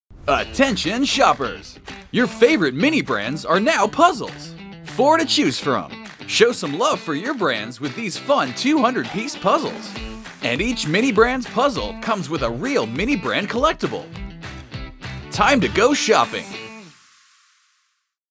Mini Brands PUZZLE SURPRISE!
Attention shoppers! You can now piece together your favorite brands with the Mini Brands Puzzle Surprise! Each one comes with a mystery collectible!

The Mini Brands line of puzzles are fun jigsaw puzzles that feature favorite food items from childhood and ones that we still enjoy to this very day. Crack open the sturdy storage tin to reveal one of seven possible puzzles. Built with high-quality materials, these food puzzles are sure to last and can be built again and again. Give your child the gift of arts and crafts with toys that foster creativity and hands-on learning.